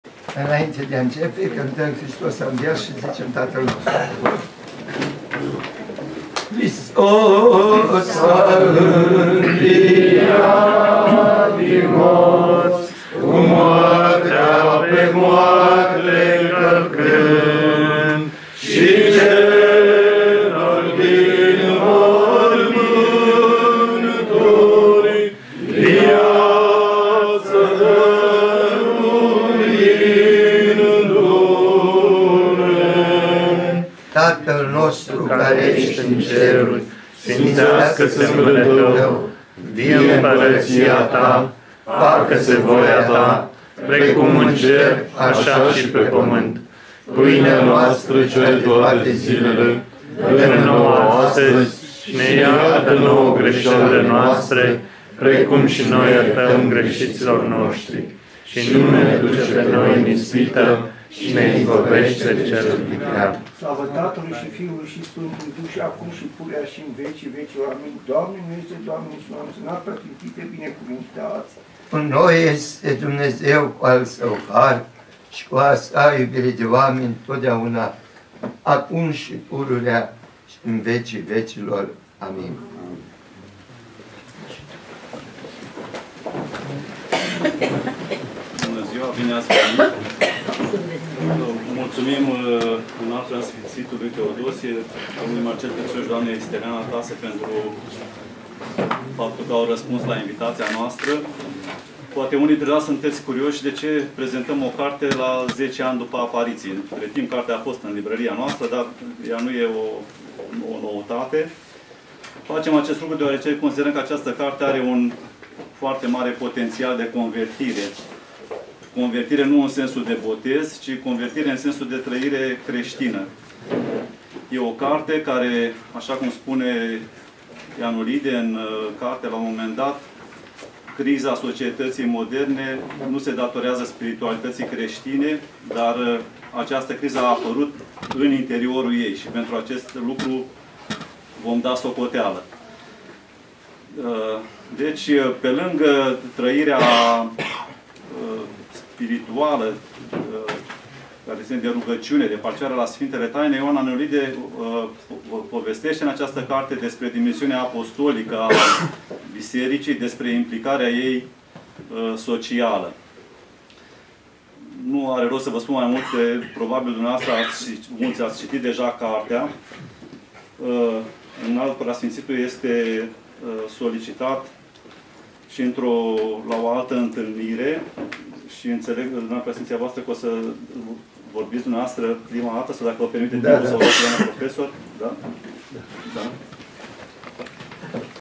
(1) Marcel Bouroș - Introducere - "Deținutul profet" - Ioan Ianolide (2018.05.10)
În data de 10 mai 2018 a avut loc la Curtea Brâncovenească (Constanța) lansarea de carte "Deținutul profet", de Ioan Ianolide. Invitați: ÎPS Teodosie al Tomisului, prof. Steliana Tase, Marcel Petrișor.

Printre participanții la discuție: ierom. Amfilohie Brânză, Alexandru Mihalcea și Gogu Cușa.

Mai jos, imagini și video de la eveniment, și înregistrarea audio a întâlnirii.